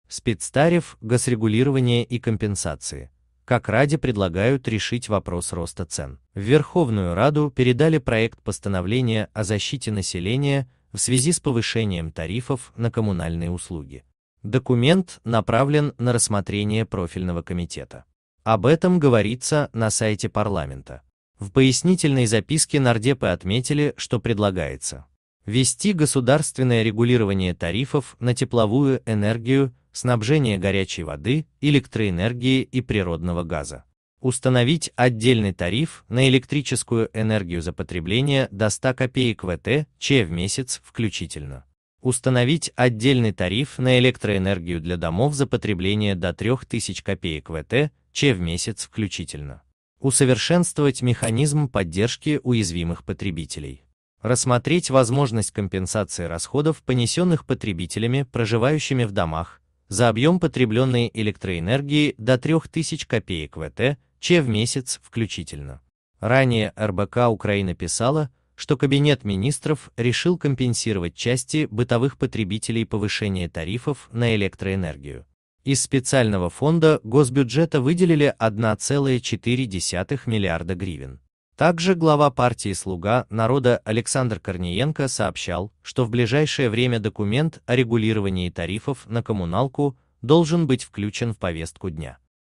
Спецтариф, госрегулирование и компенсации: как Раде предлагают решить вопрос роста цен.
Политика тарифы ВерховнаяРада Подробней на В Верховную раду передали проект постановления о защите населения в связи с повышением тарифов на коммунальные услуги. Документ направлен на рассмотрение профильного комитета. Об этом говорится на сайте парламента. В пояснительной записке нардепы отметили, что предлагается:. ввести государственное регулирование тарифов на тепловую энергию, снабжение горячей воды, электроэнергии и природного газа;. Источник